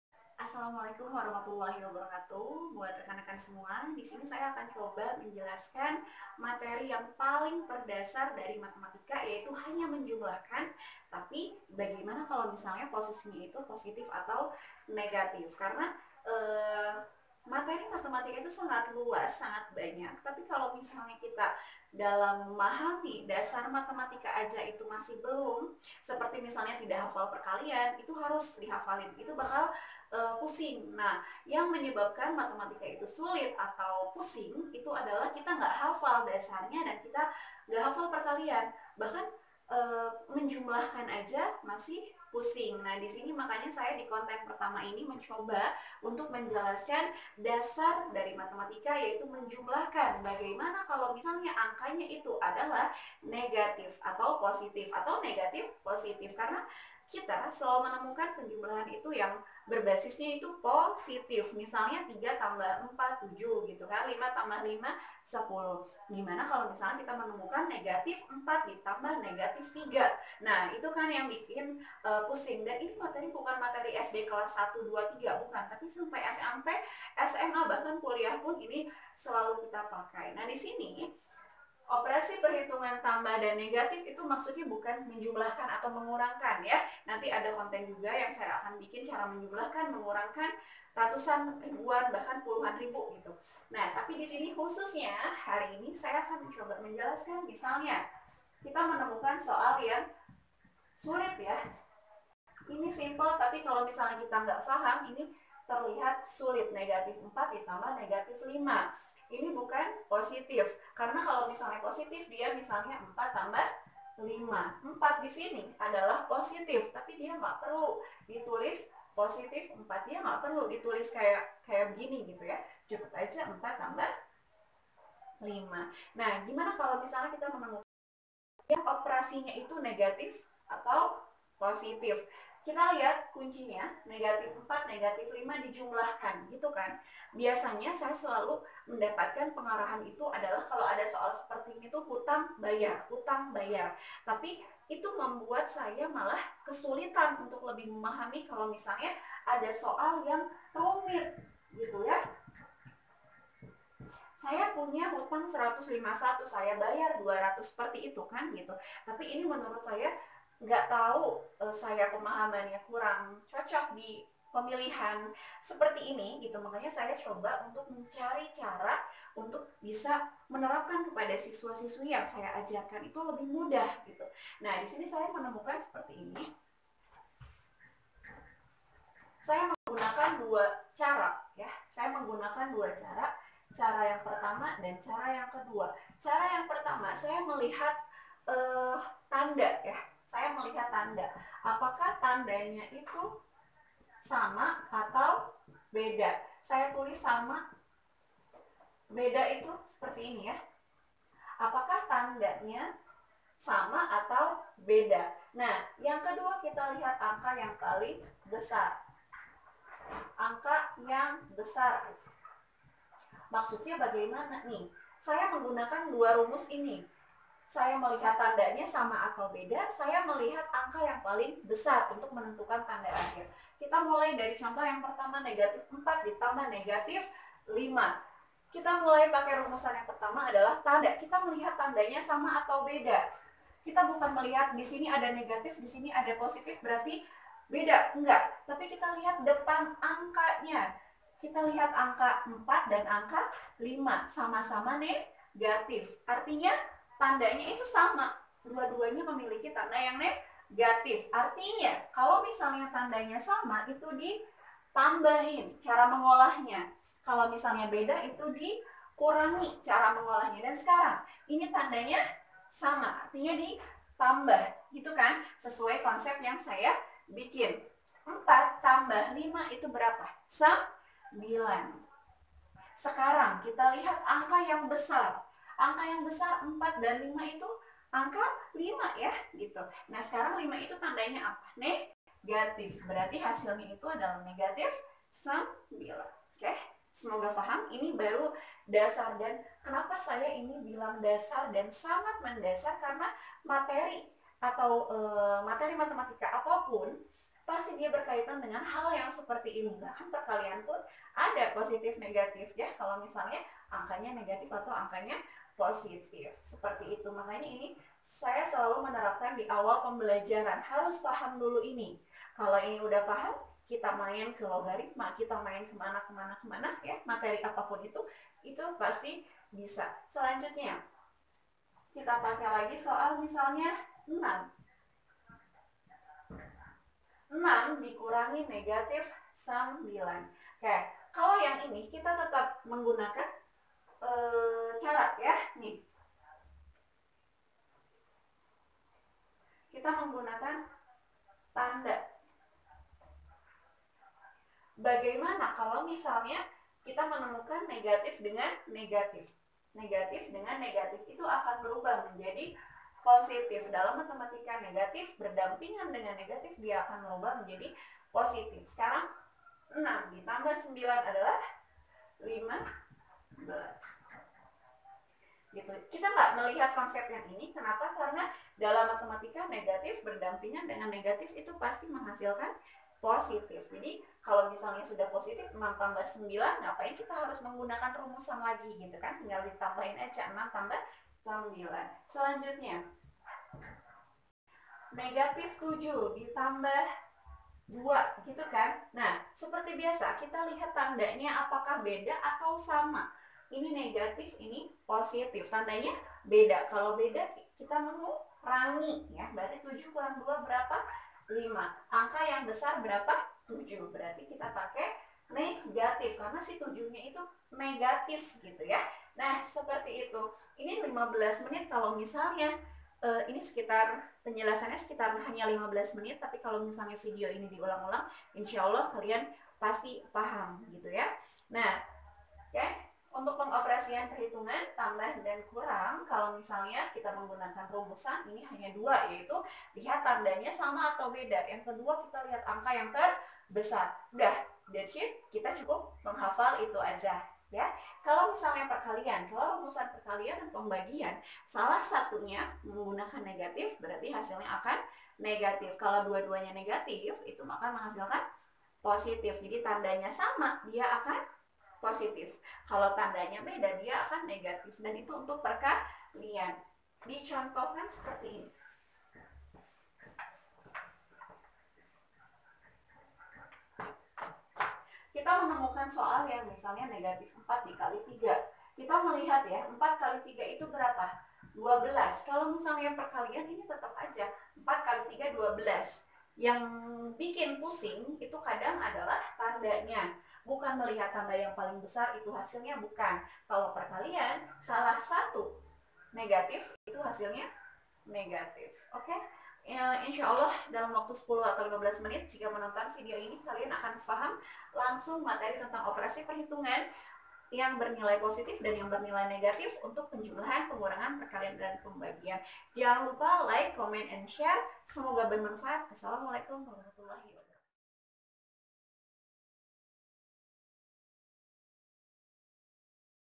MATERI1 : OPERASI HITUNG BILANGAN POSTIF NEGATIF (CEPAT PAHAM DALAM 5 MENIT)
Di Video kali ini, yang sudah sekian lama saya vakum tidak membuat video lagi, saya memohon maaf jika ada kesalahan dalam berkata atau berucap.

Semoga video ini bisa membawa manfaat dan bisa dengan mudah dipahami oleh rekan rekan semua dan adik adik semuanya...